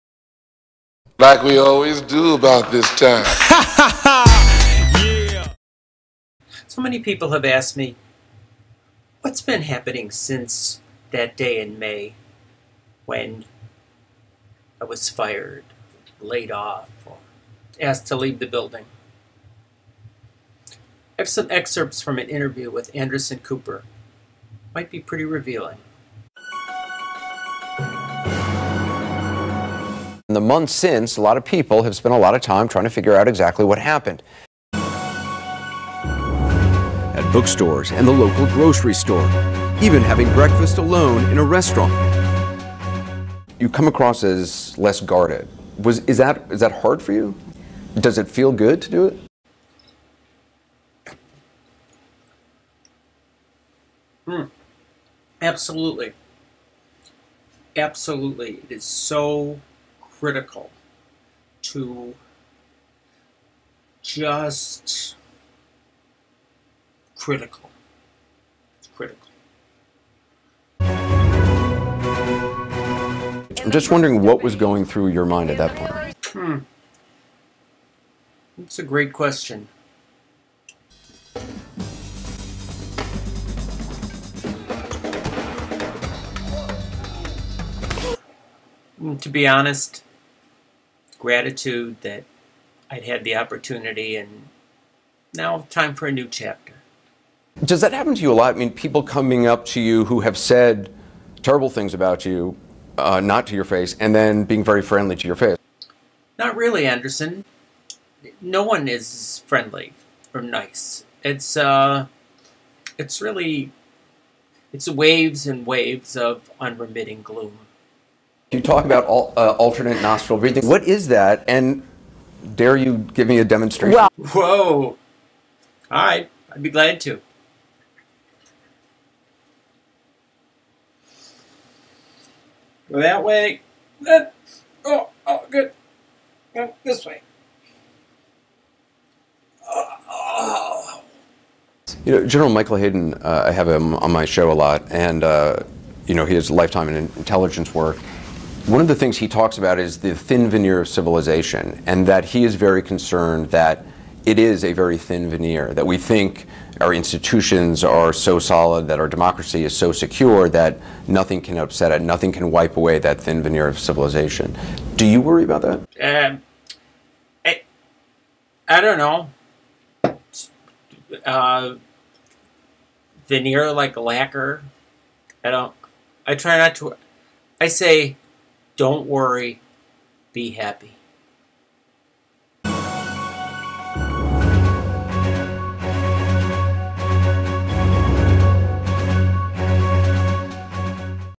Anderson Cooper Interview with Don's Basement
Don is revealed by the incisive questioning of Anderson Cooper